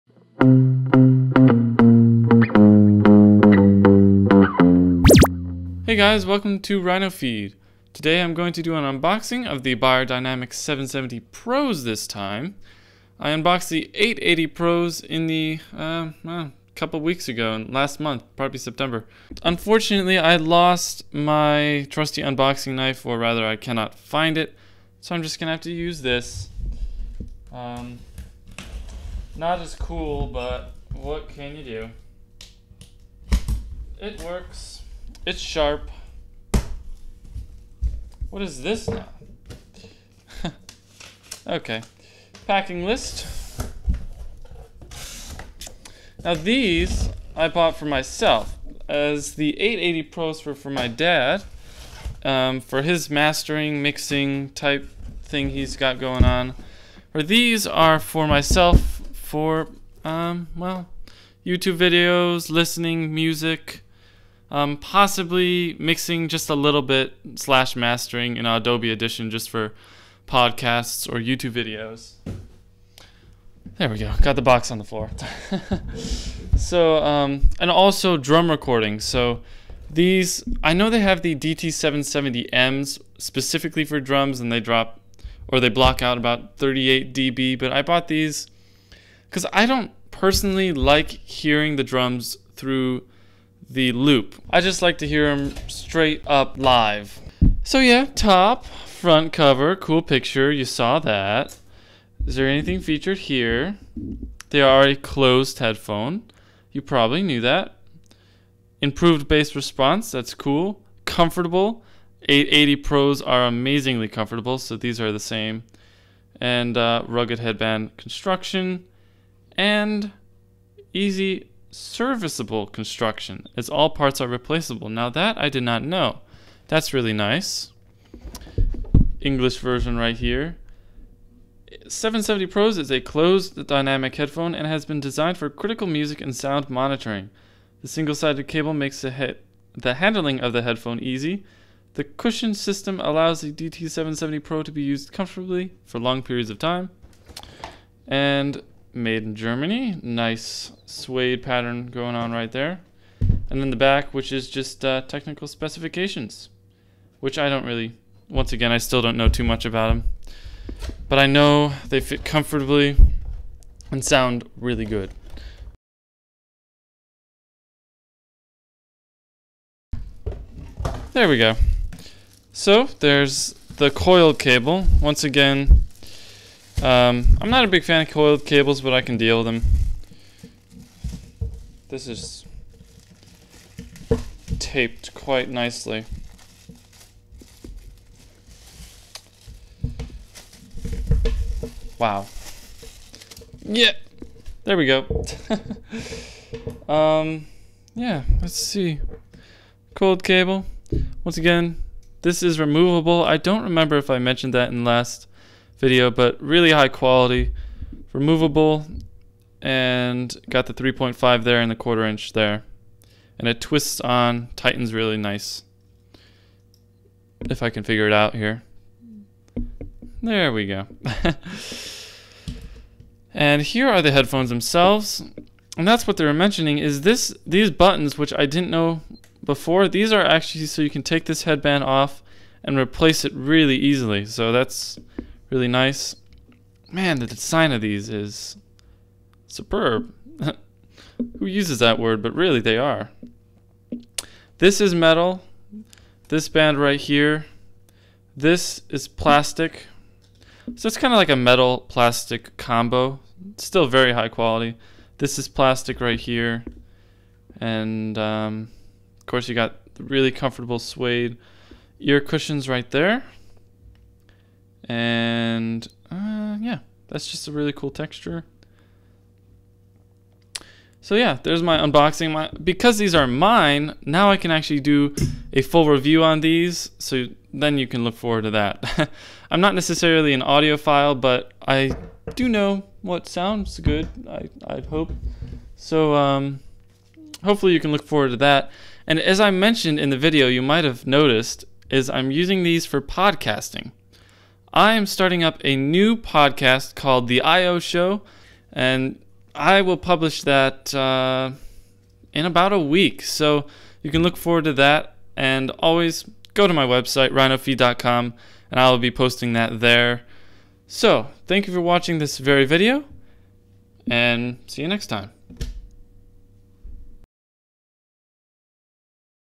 Beyer Dynamic DT770-Pro Headphones Unboxing
Here we take a look at the DT770 Pros this time! Look forward to a full review in the future.